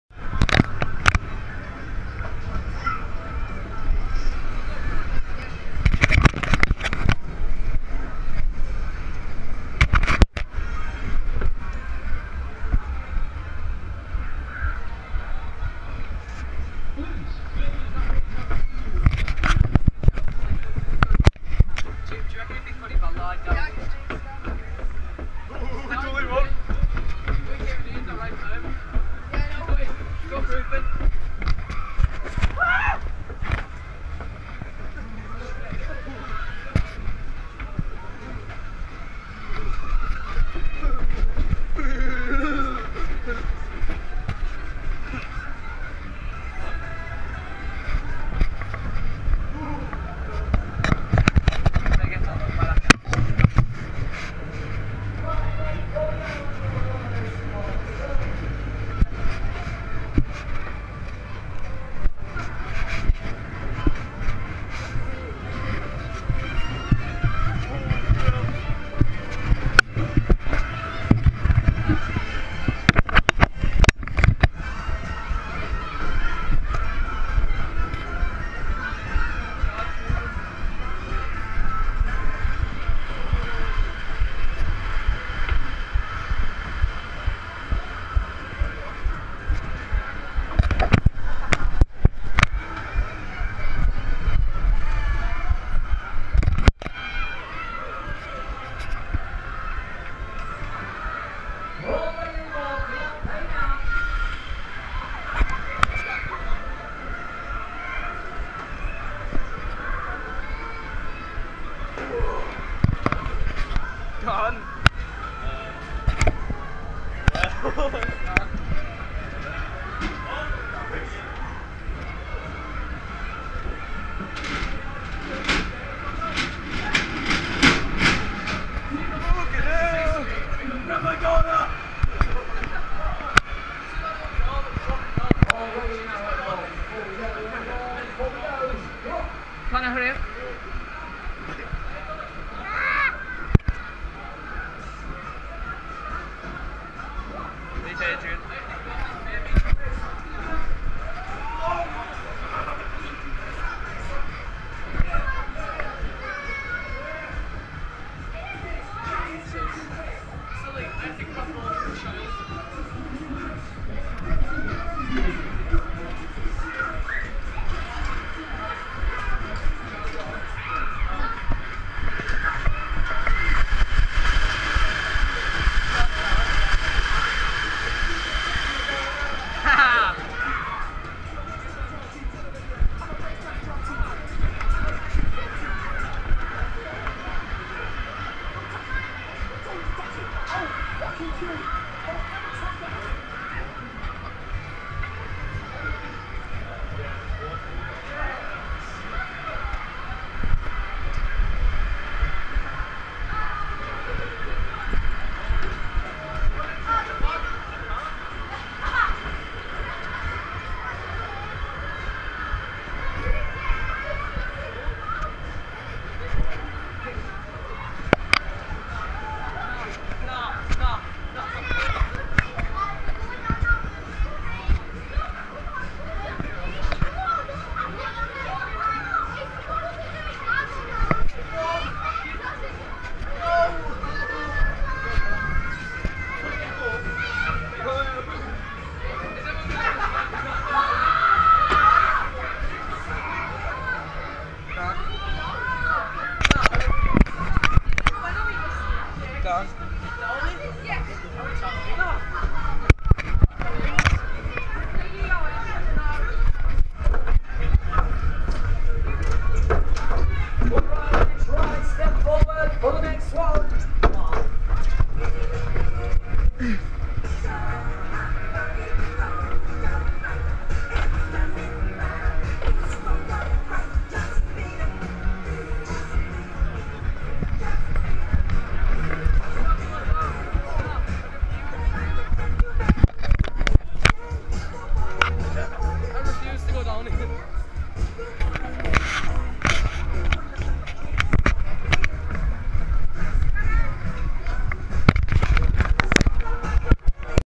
The fun house / Whitehaven festival 2013
This was shot with my gopro hero 3 white edition and show me going through the fun house. A pit is just black because I had to cover up my camera as there was a owner watching me.